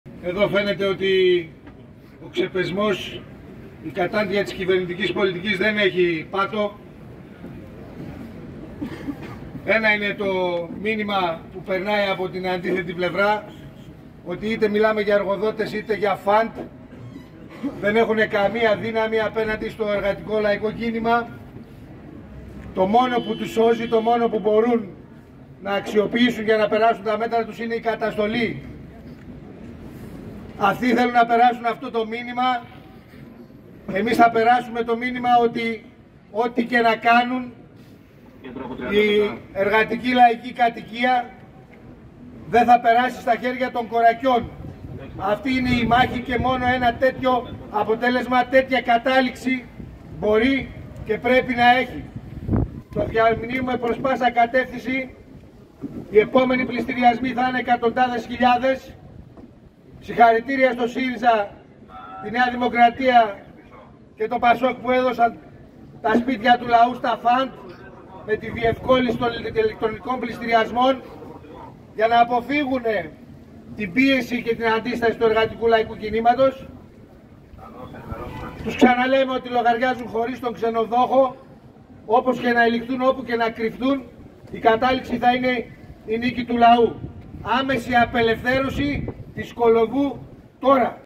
Λ. Στολτίδης: Να απελευθερωθεί άμεσα η Ιωάννα Κολοβού τώρα
Την άμεση απελευθέρωση της χαμηλοσυνταξιούχου Ιωάννας Κολοβού, τώρα, ζήτησε ο Λεωνίδας Στολτίδης, βουλευτής του ΚΚΕ, έξω από το ΑΤ Ζωγράφου.

«Ό, τι και να κάνουν, η εργατική λαϊκή κατοικία δε θα περάσει στα χέρια των κορακιών» τόνισε, επισημαίνοντας ότι «λογαριάζουν χωρίς τον ξενοδόχο».